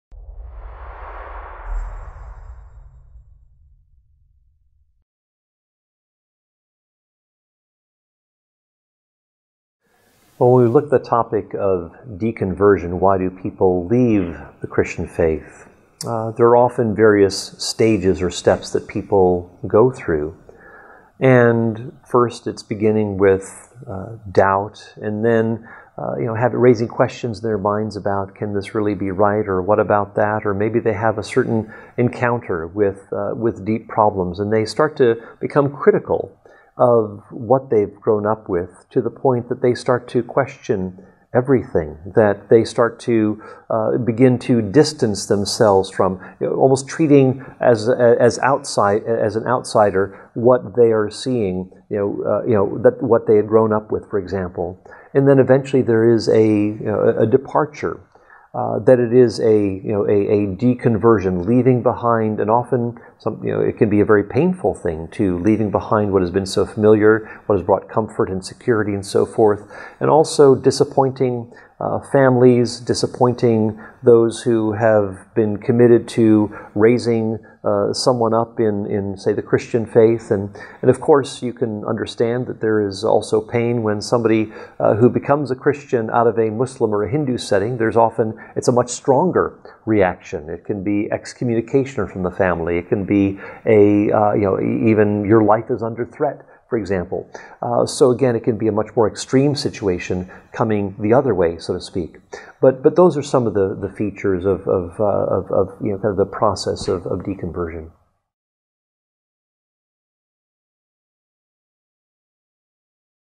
What does it mean to de-convert from the Christian faith?
Paul Copan, Chair of Philosophy and Ethics, Palm Beach Atlantic University, answers the question "What does it mean to de-convert from the Christian faith?" related to his talk "De-Conversion: Why People Leave the Christian Faith - and (Re)Turn to It" given at the 2018 European Leadership Forum.